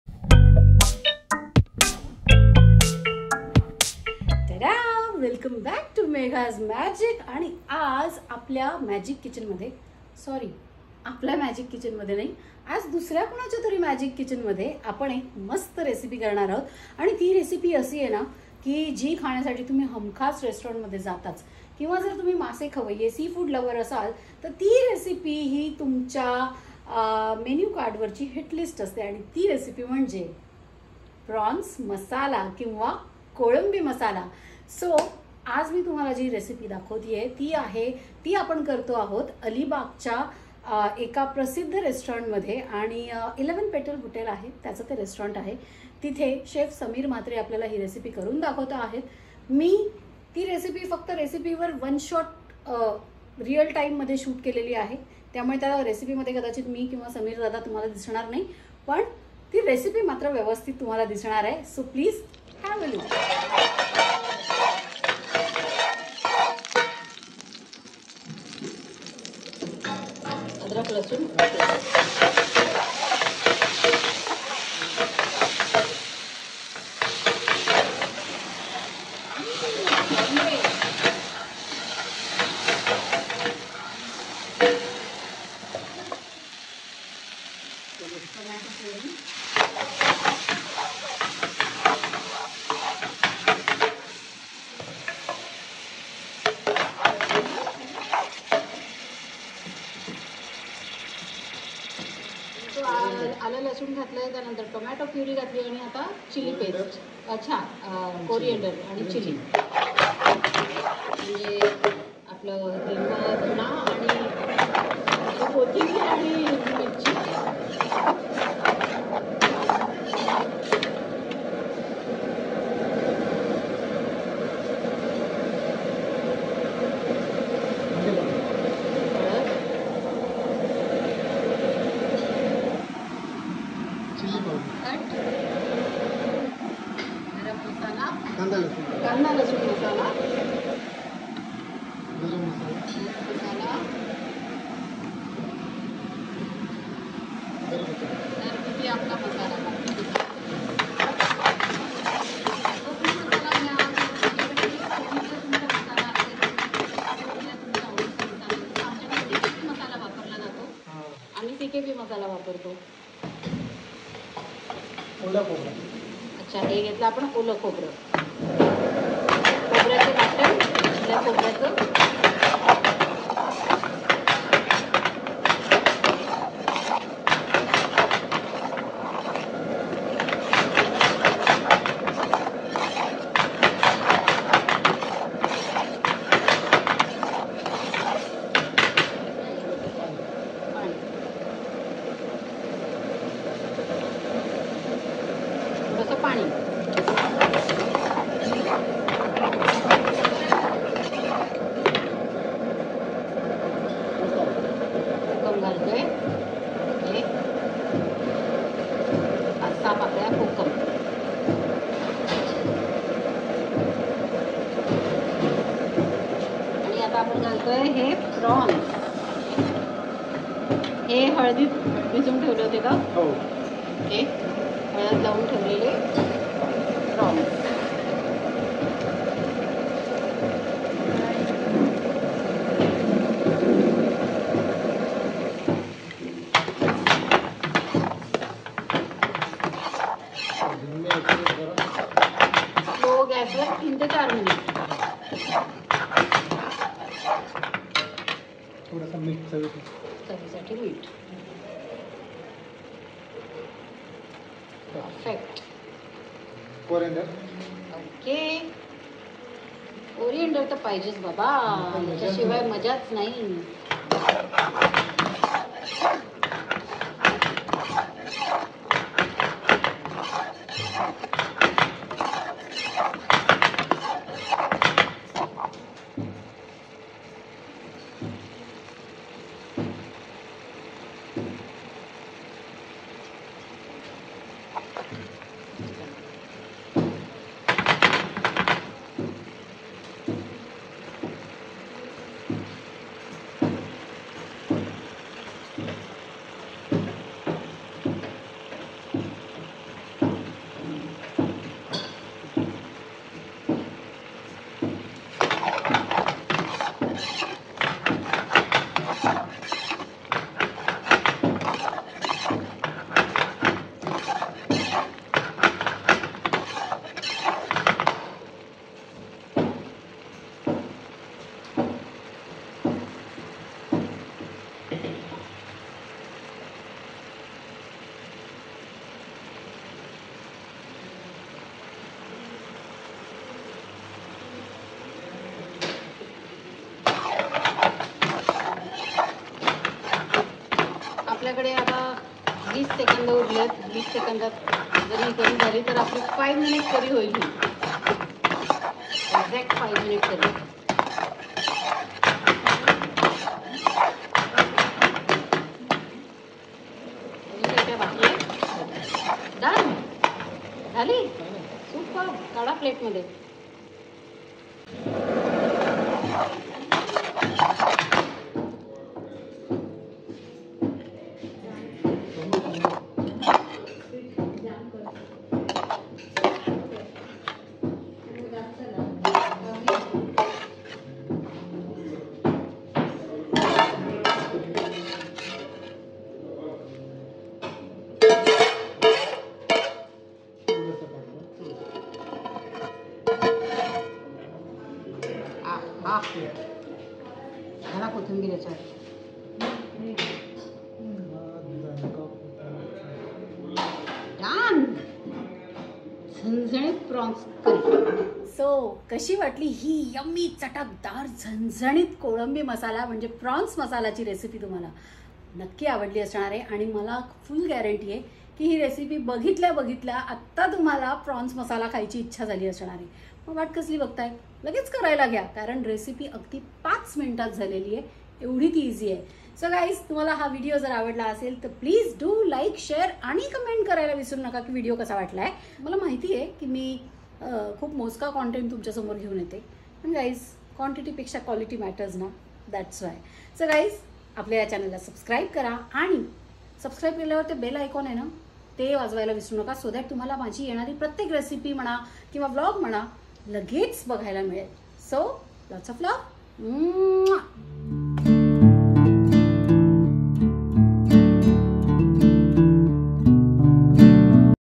Restaurant Style Prawns Masala Curry in 5 minutes | कोळंबी मसाला।Megha’s Magic
Location and Recipe: Eleven Petals Resort Alibaug/ Nagaon
 Chef : Sameer Mhatre